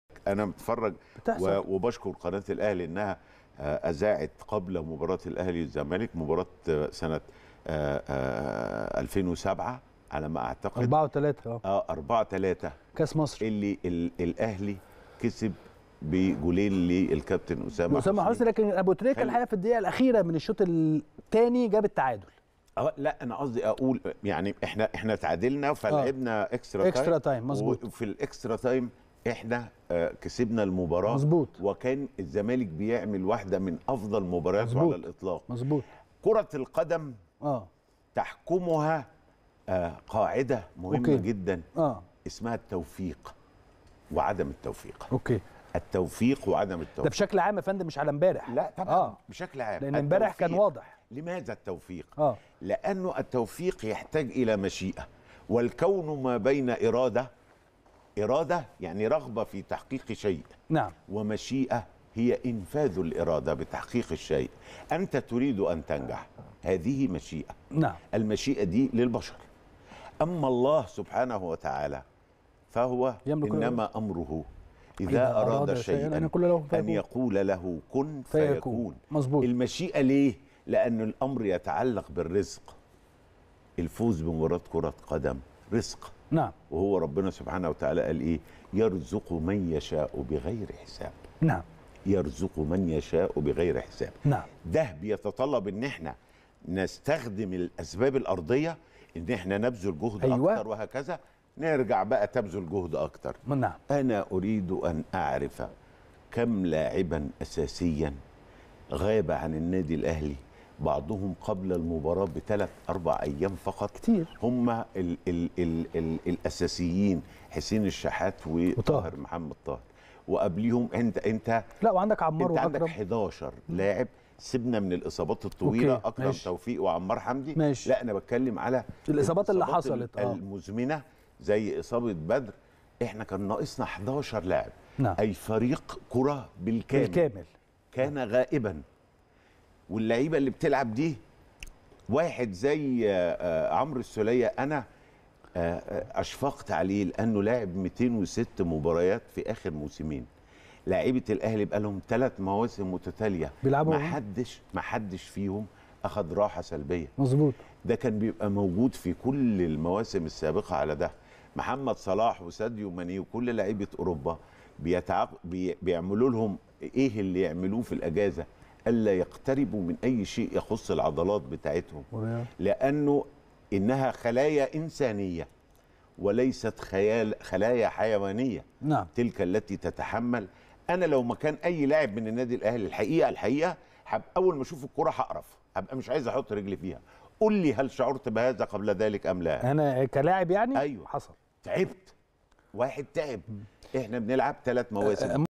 عز الدين: رفقًا باللاعبين تراكم المباريات أجهدتهم.. كم لاعبًا أساسا غاب عن الأهلي بسبب "الإصابة"؟
حامد عز الدين: رفقًا باللاعبين تراكم المباريات أجهدتهم.. كم لاعبًا أساسا غاب عن الأهلي بسبب "الإصابة"؟